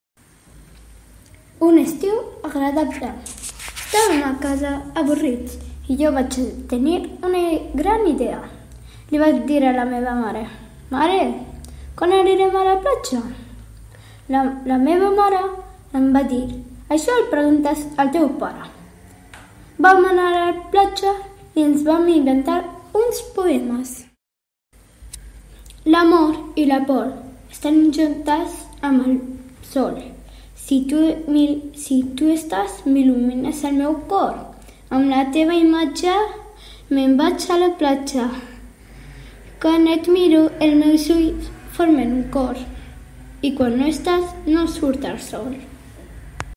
Un estiu agradable
Poema realitzat per la Sara de 5èA de l'Escola Montessori de Rubí. Curs 2013-14.